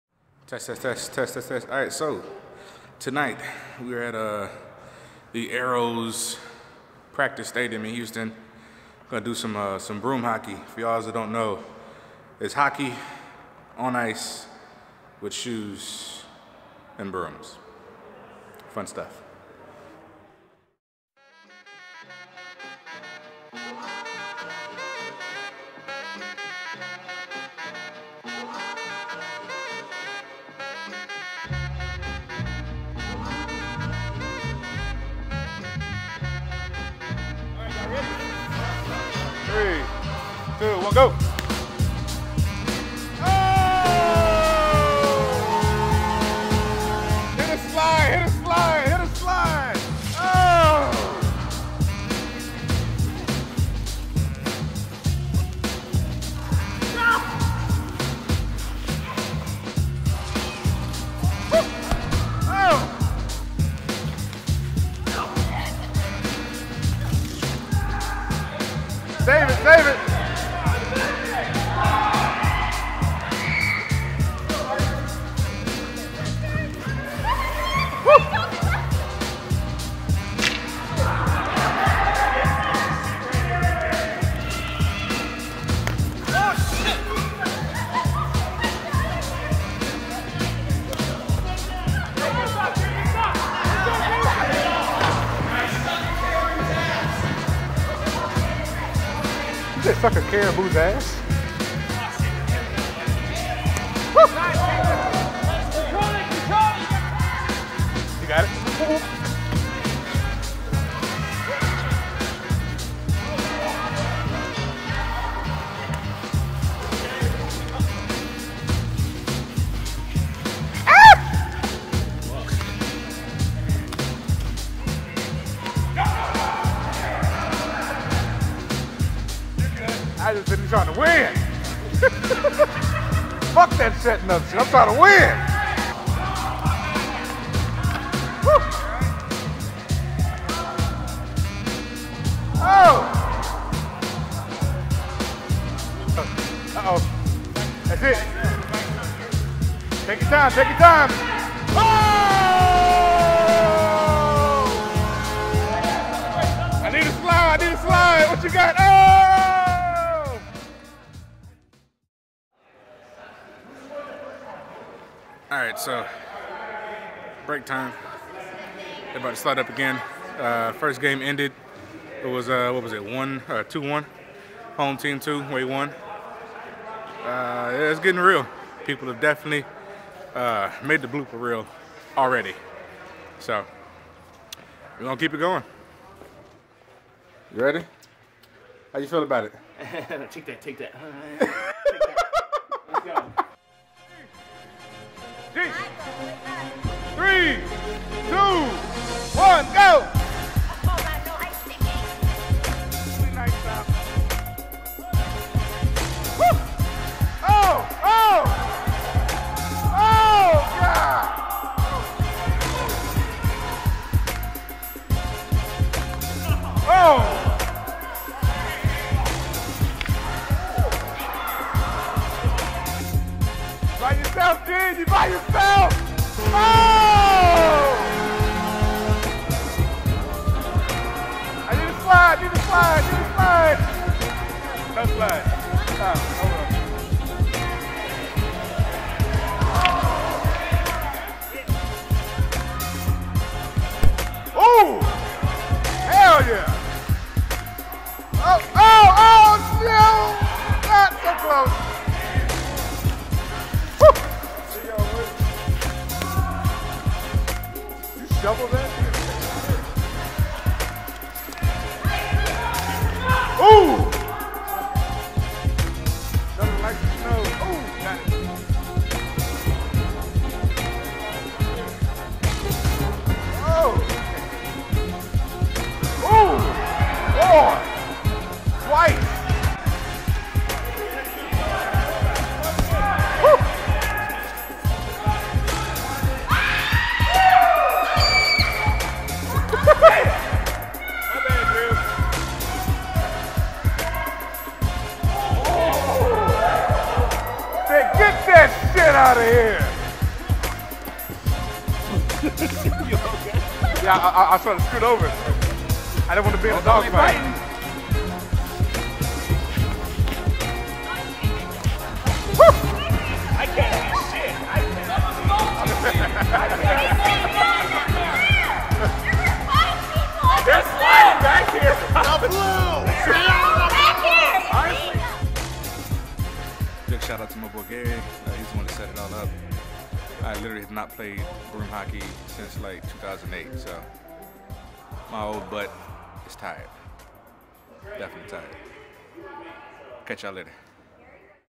Broom Hockey Highlights
A few of us quidditch folks and friends of ours got together to try something most of us haven't done. It was a ton of fun and no one was seriously hurt. Something everyone must do at least once.